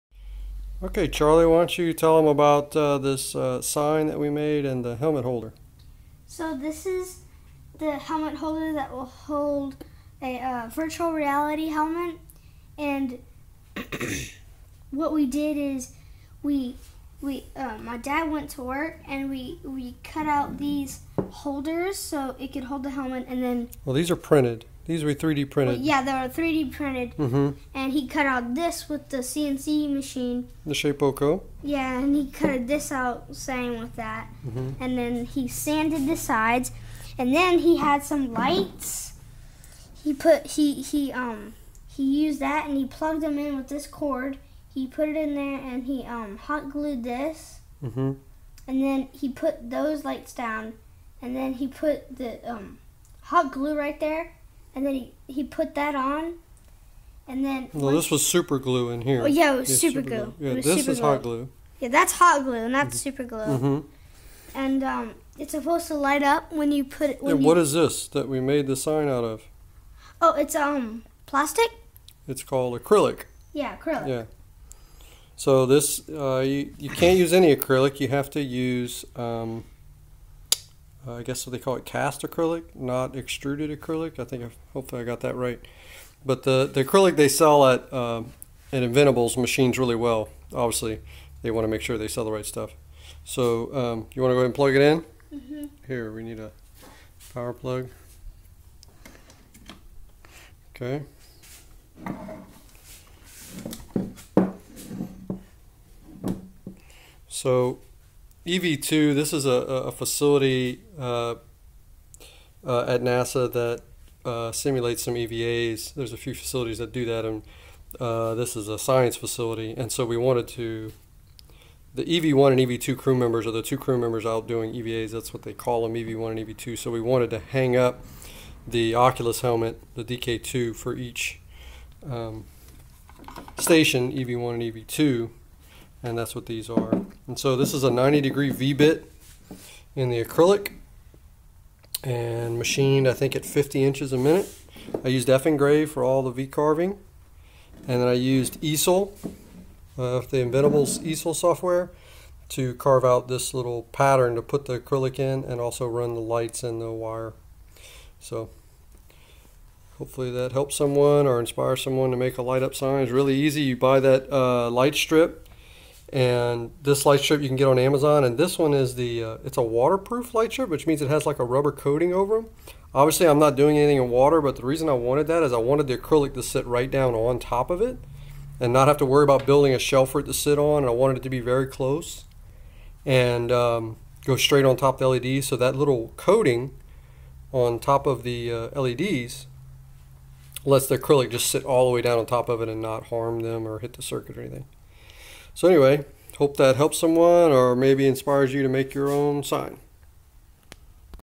CNC LED Acrylic Sign for Oculus Holder
My son and I made some holders for the Oculus DK2 VR helmets which we were using at work.  I decided it would be a good opportunity to learn how to mill acrylic to make an edge lit LED sign.  After a few test pieces, we were able to get decent results.

The acrylic is from inventables and you can get it

Also, if you want to mount it in walnut like I did, you can get that from inventables

You can use any LED strips you want.  Amazon and Inventables both carry tons of options.  If you want to use the same ones as me (I wanted mine to be powered off of a USB port), I used these

The actual Oculus holder pieces were 3d printed and the design is available